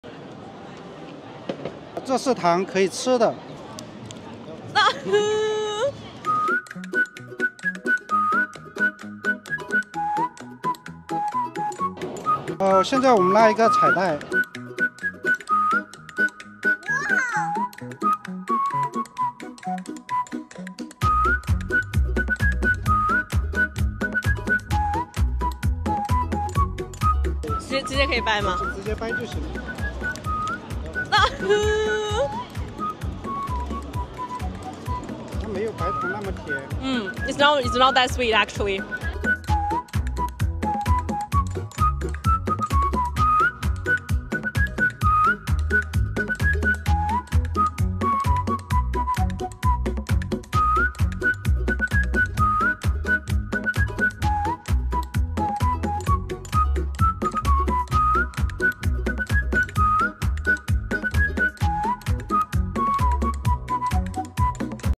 Confectionery at Belt and Road International Skills Competition
Look at these desserts and cakes! Teachers and students from pastry and baking art schools showed off their exquisite confectionery during the Belt and Road International Skills Competition.